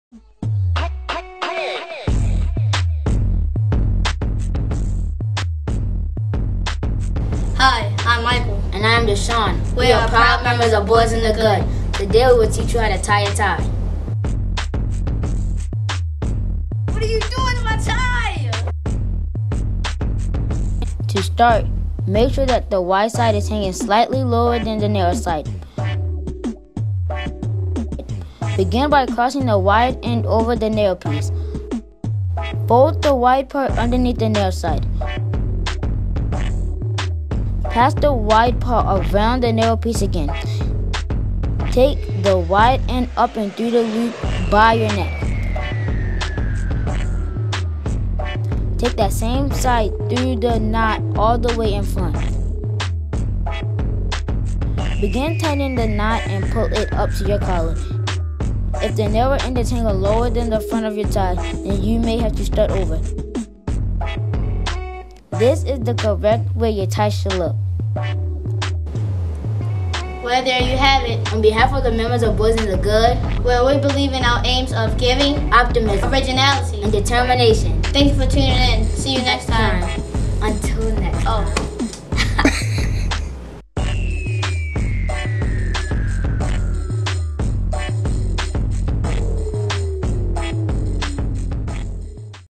"Boys in the Good" Presents... How to Tie a Tie
Learn the quickest and easiest way to tie a necktie. Michael and Deshaun will show you simple step-by-step instructions to tie the four-in-hand knot.

to learn more about the "Boys in the Good" Youth Leadership Program.